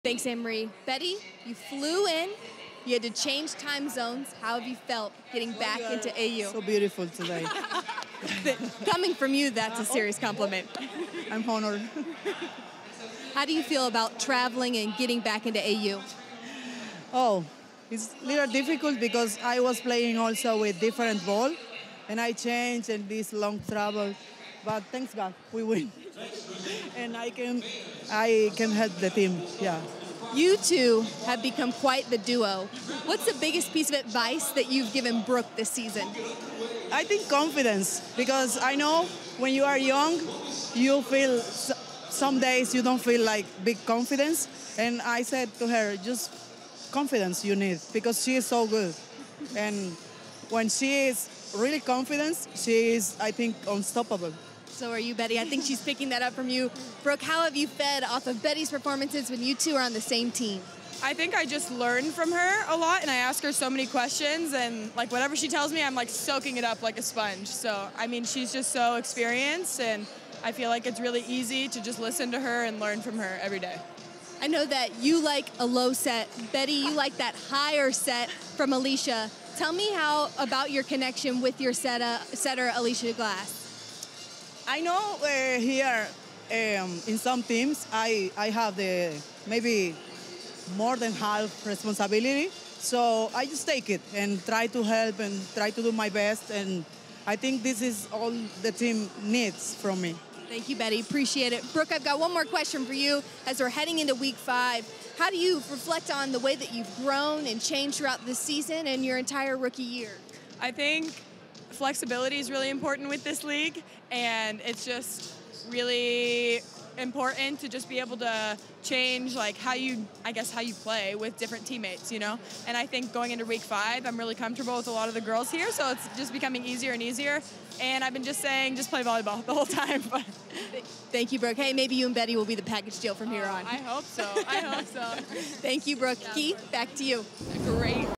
Savanna Collins Post Game Interview: Brooke Nuneviller and Bethania De La Cruz
The dynamic duo of outside hitters Brooke Nuneviller and Betty De La Cruz combined for 31 kills and 17 digs to guide Team Hentz to a 79-68 victory 👏